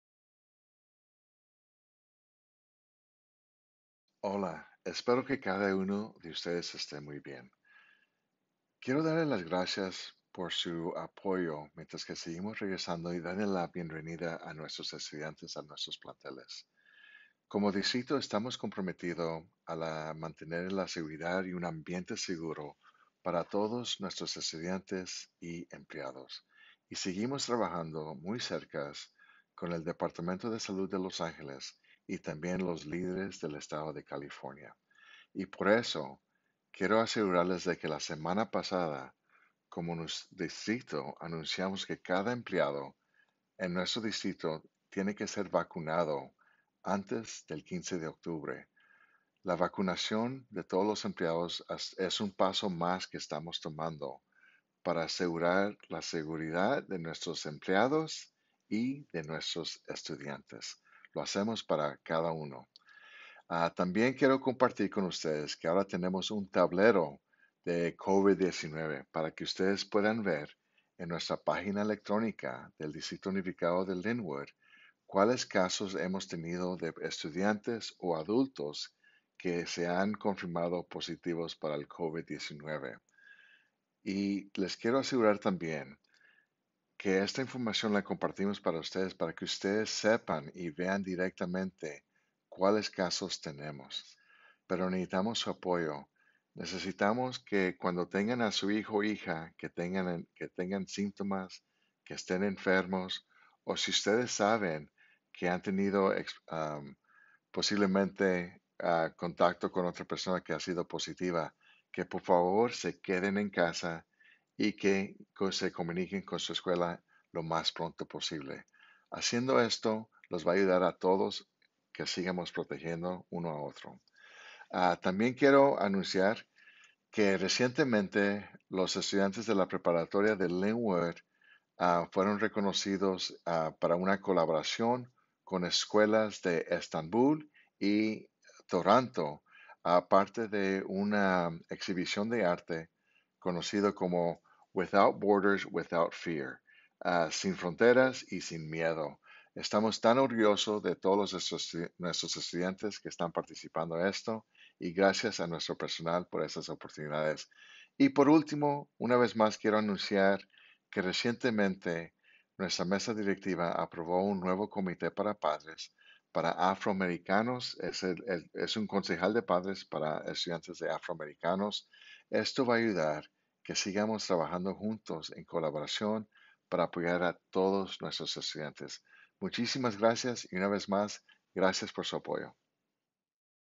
Actualización del Superintendente 09.02.21
El Superintendente Crosthwaite proporciona las últimas actualizaciones del distrito.